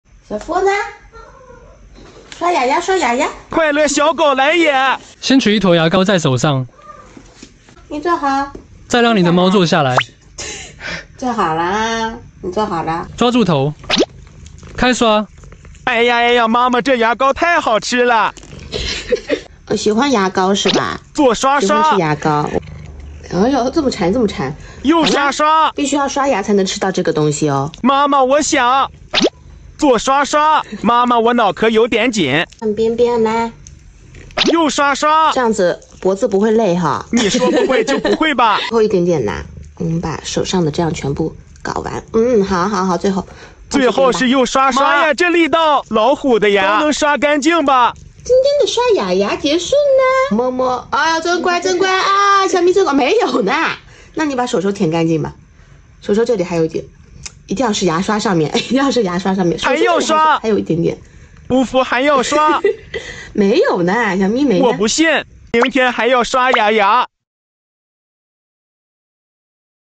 Brushing Teeth? It's That Easy!
Hey there! In this video, I'll be showing you just how easy it is to brush your teeth! From choosing the right toothbrush to mastering the perfect technique, you'll become a pro in no time. Let's get those pearly whites sparkling together!